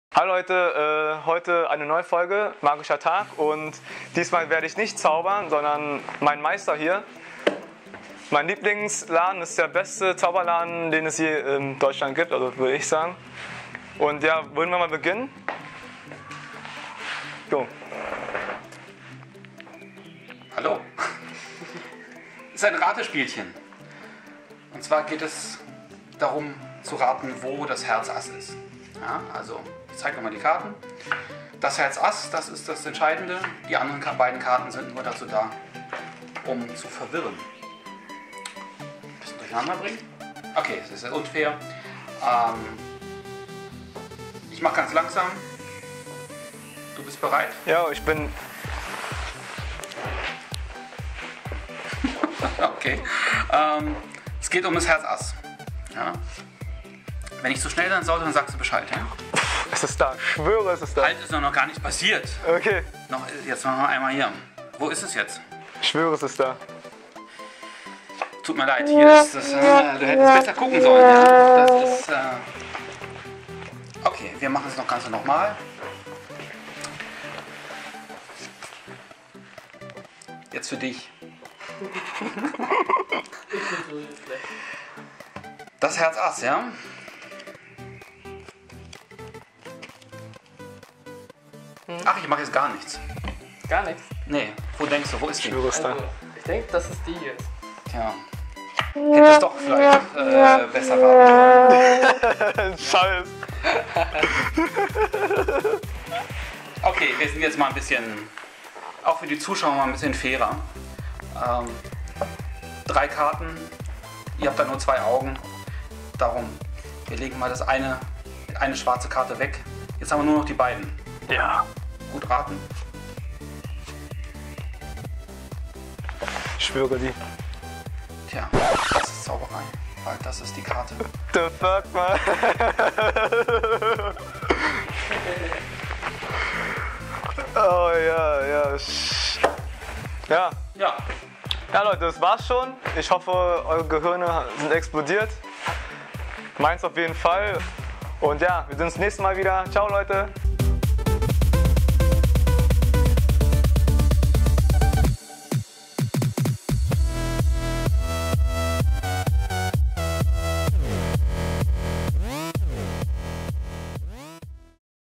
Magischer Tag #2 - Mein Sensei zerreißt !
Hey ihr Lieben ! Letztens habe ich wieder mal mein lieblings Zauberladen besucht
und mein Master zeigt euch einen kurzen aber sehr coolen Zaubertrick :) Viel Spaß !

Wenn du Interesse an Magie hast, dann schau hier vorbei:
Adresse:
Fabiolus Magic Store
Frankenstrasse 13
10781 Berlin